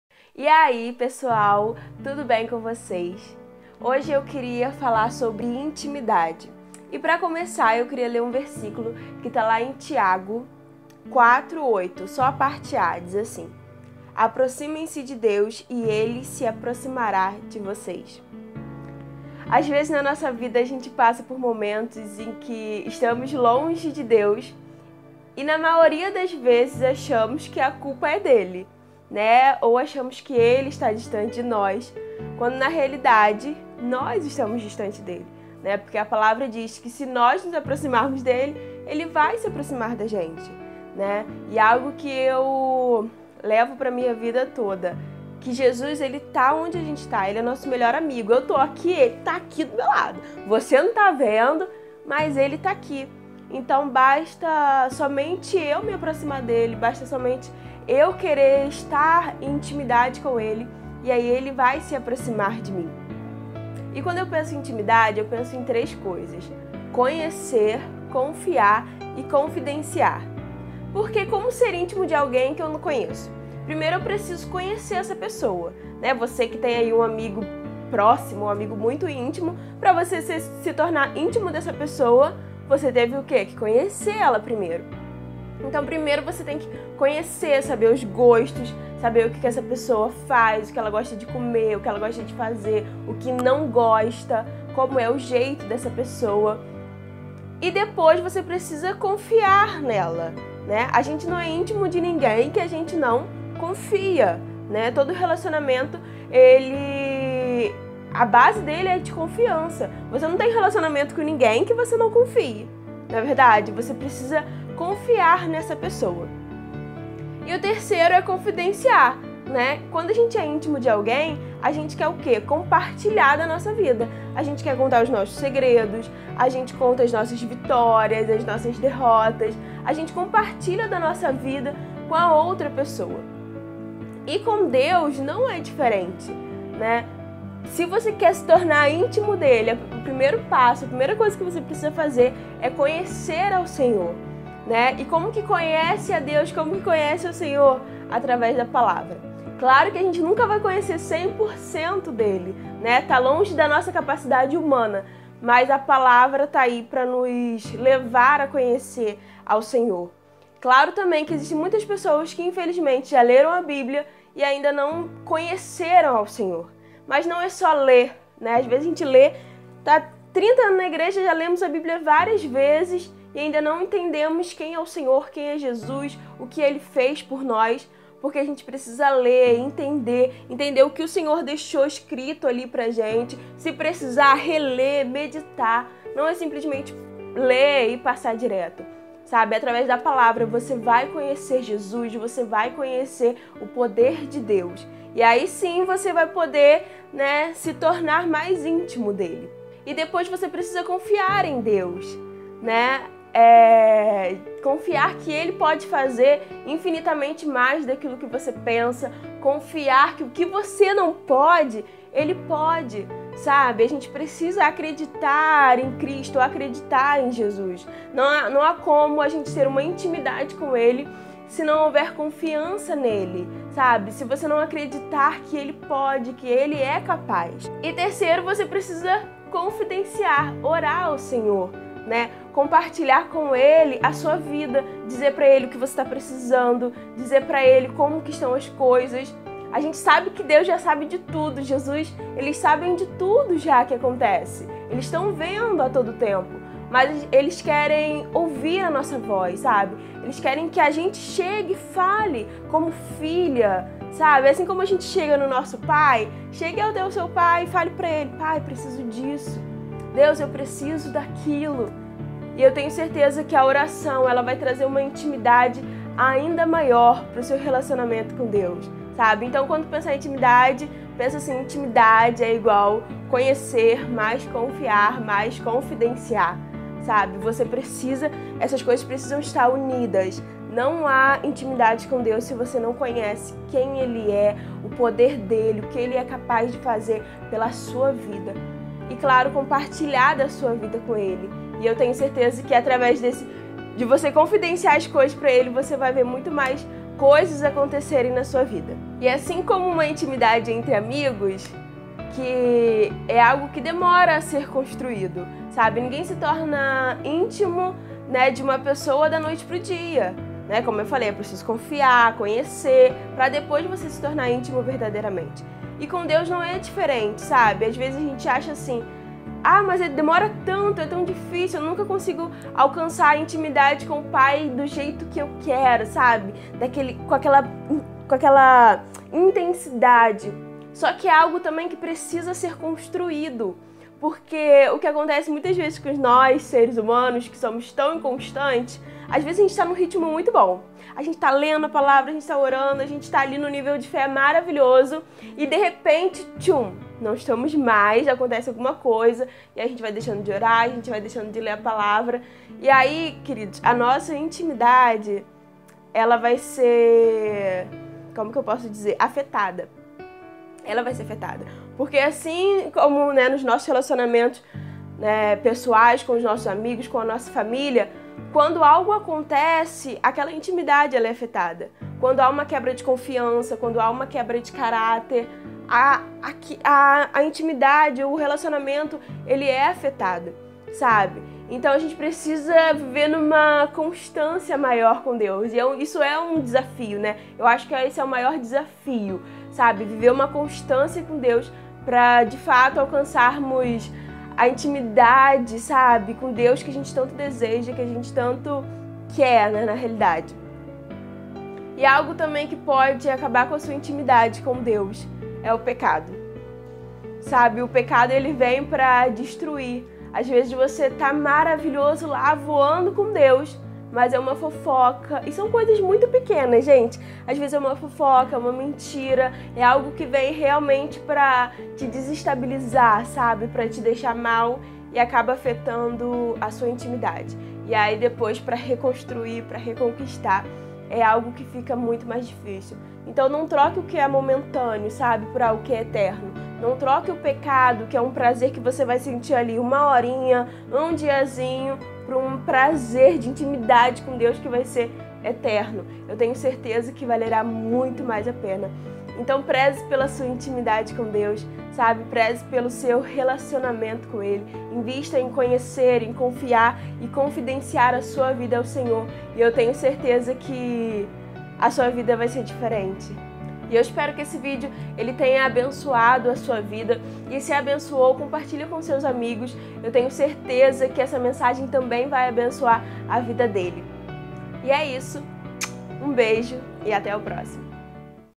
COMO TER INTIMIDADE COM DEUS | Larissa Colombino
Intimidade com Deus é fundamental em nossa vida cristã. Compartilho nesse vídeo um pouco sobre como ter intimidade com nosso Deus e como isso muda nossas vidas.

Conta também para mim nos comentários sobre a sua experiência de intimidade com Deus.